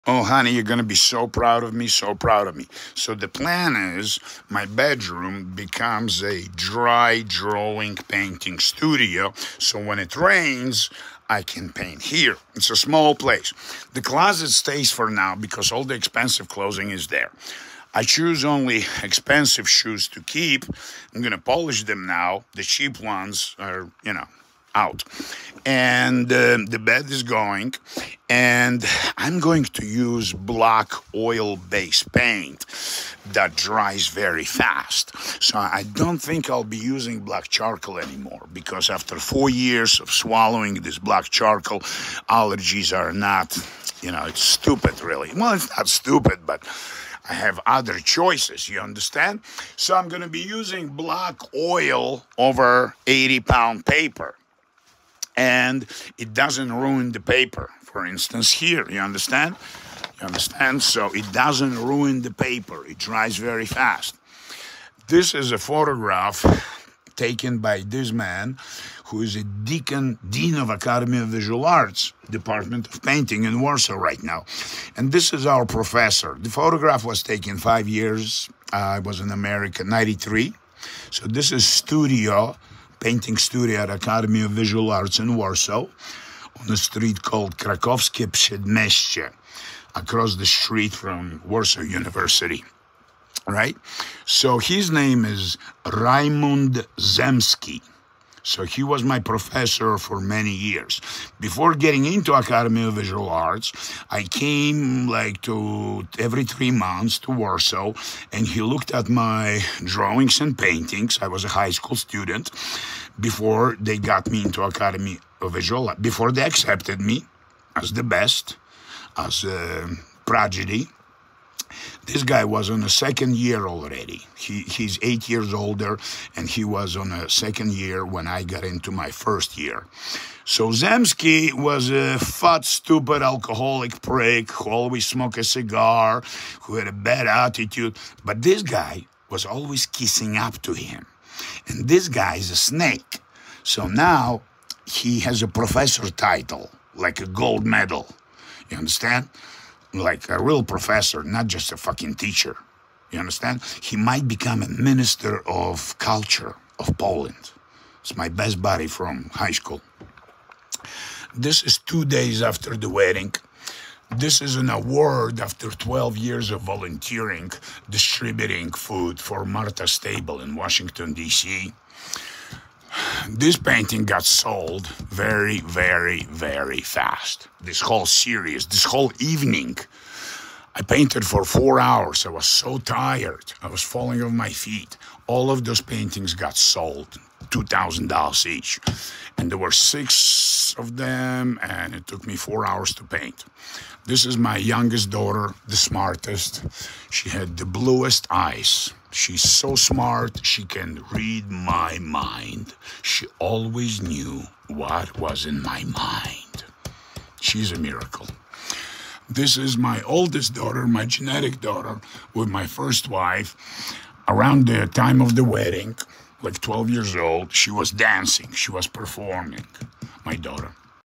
April 4, 2024
This is how I want my life (my immediate environment/reality to be).
My home (temporary location, as usual) has (must have) no
 inside, or outside. Porch, view on a bay, 60 yards from big body of water, 1 hour in a Subway ride from MOMA -“museum of modern art”53th.street).
Everything inside my living space(blessings from this quantum universe)is a work in progress.(as my life always is)
Photographs from years ago, are supposed to inspire beautiful memories to create epic symbols (of love, compassion, wisdom, sharing resources, anger, addictions,with relationship to myfuture !
The paintings , drawingsketch hanging on the walls are being stored, reviewed, improved, offered for sale, repainted and rehang continuously.
Food (+preparation Ayurveda) takes no more than 3% of a square footage of my house. My sleeping 🛌 area must be no more than 5% of my space. My clothing is in the closet and a drawers (most of my clothing I don’t wash I throw it away after a few days, only most expensive suits).
My couch is occupied by worksinprogress in a daytime.
every square inch of my ceiling is being utilized for smokingtechnology (black sued (копыть копчение) from candles or burning oil) for drawings.
To remind you: it’s a 40.000 year-old drawing technology developed by caveman in Europe. Do you understand the philosophy of how I manage my spacetime, my life?
-“Little food, little sleep, continues creation and sex” my model for life right now after the divorce.
I was given years of livelihood on this planet we call it privacy intimate pace aka
let’s say I have 90 years of time but I was also given space. In this case myspace is an expression of the inside of my skull 💀 mylove my quantum mind, my infinite mindsetcoach ( just as I was trying to create my whole life, since high-school). My house is what I’m thinking about all day obsessed with creation of space-time a.k.a my destiny and this is what I do all day long.
I produce infinite timeless artworks and this is the only purpose of life my existence. I have no other logical reason to wake up in the morning besides
Bogoslowsky Богословский Андрей